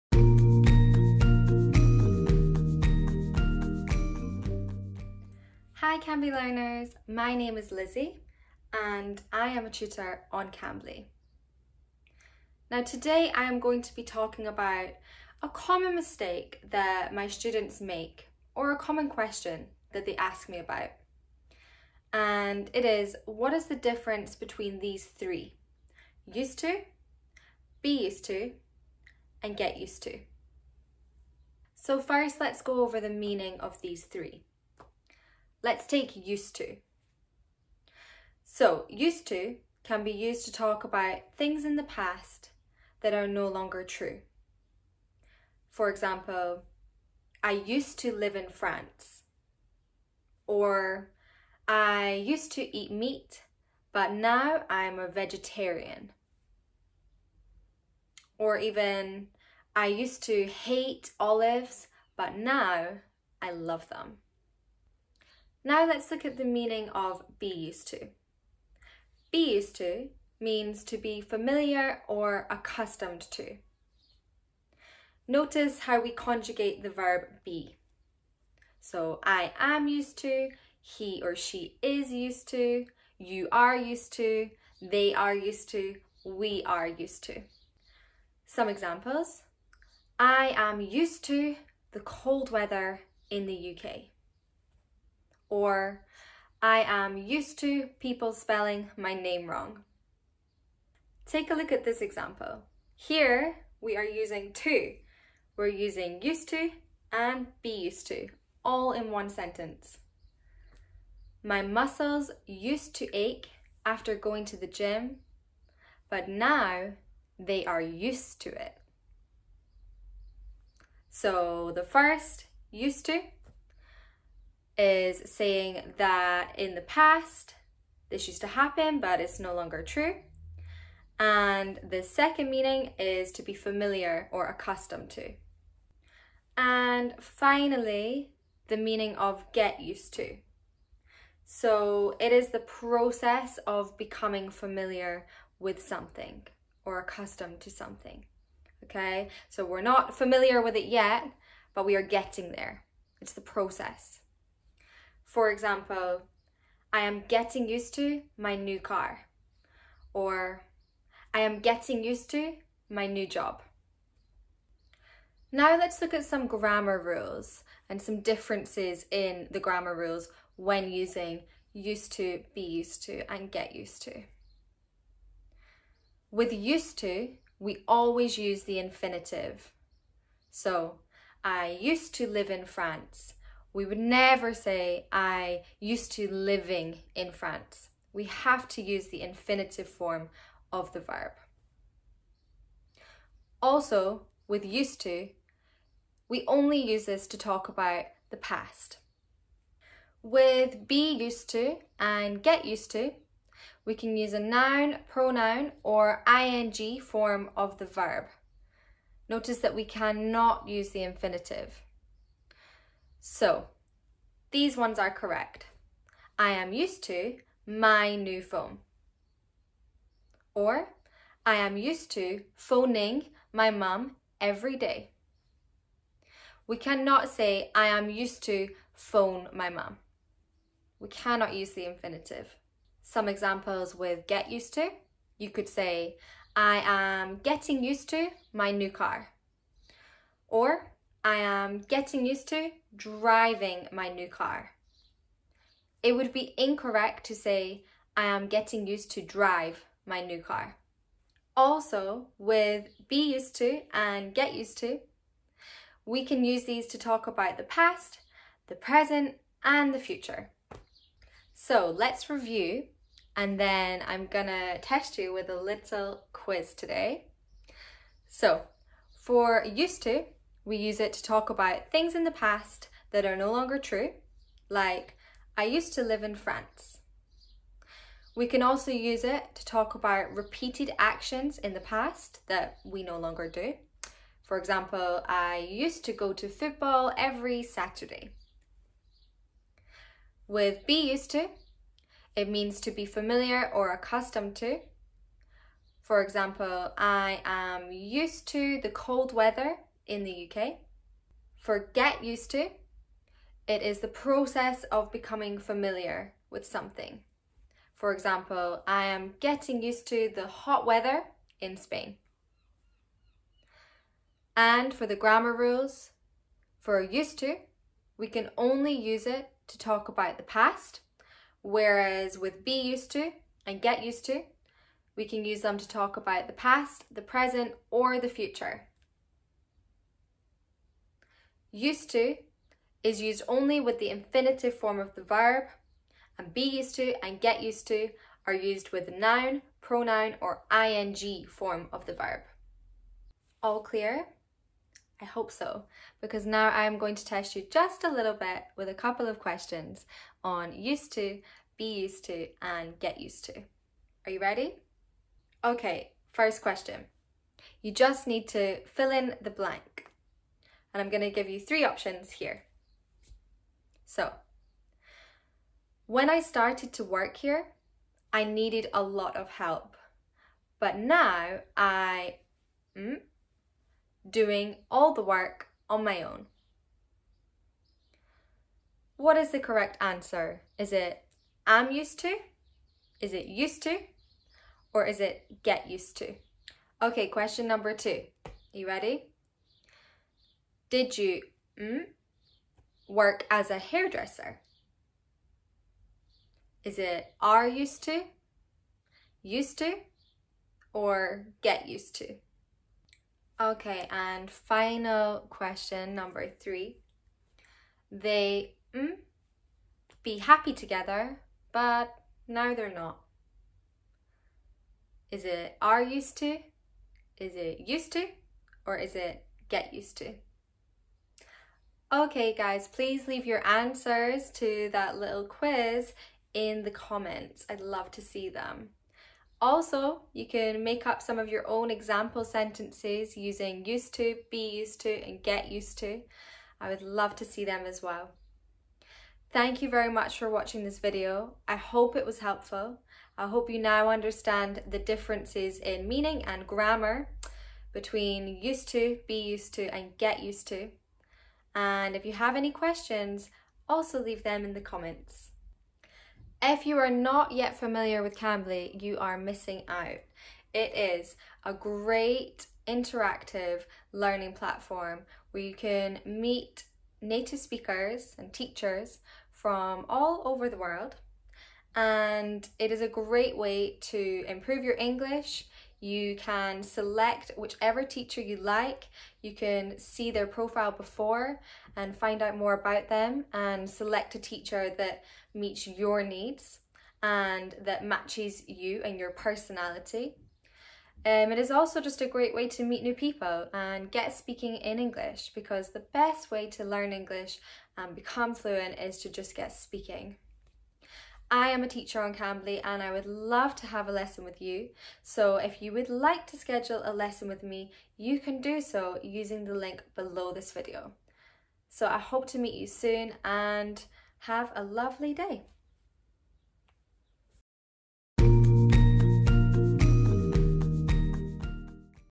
Used To vs Be | Get Used To
In this lesson, learn how to use the phrases – used to, be used to & get used to – in the correct way.